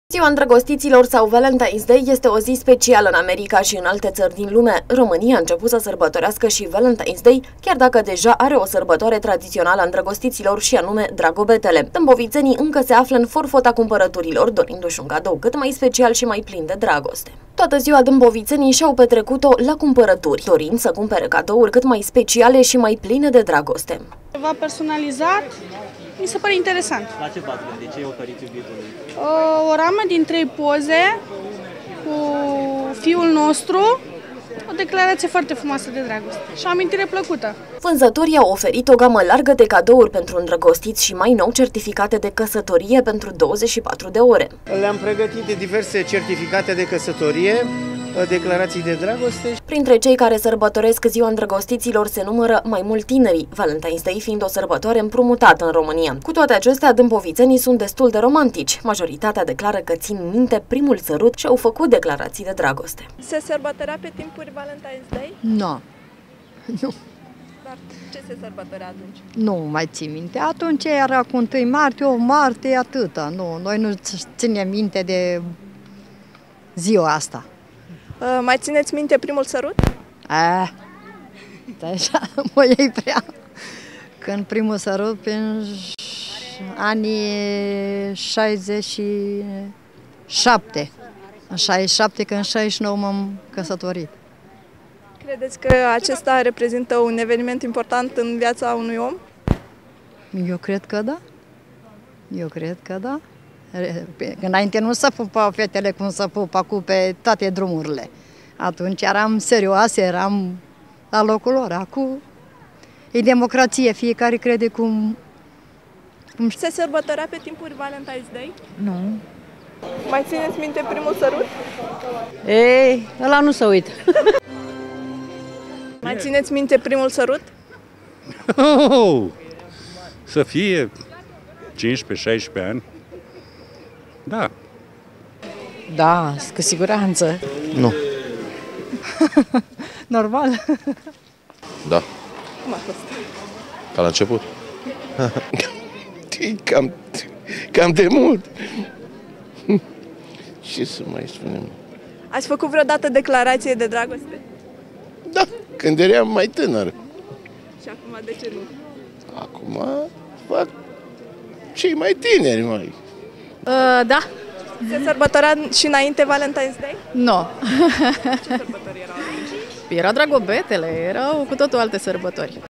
Valentine's Day sau Ziua Îndrăgostiților (Columna TV)
Valentine's Day este Ziua Îndrăgostiților în tradiția occidentală, care este celebrată pe 14 februarie. De obicei, Valentine's Day se sărbătorește oferind ciocolată, flori, diverse cadouri prin care persoanele își exprima dragostea. În România sărbătoarea specifică îndrăgostiților este Dragobetele, dar în ultimul timp din ce în ce mai mulți oameni sărbătoresc și Valentine's Day.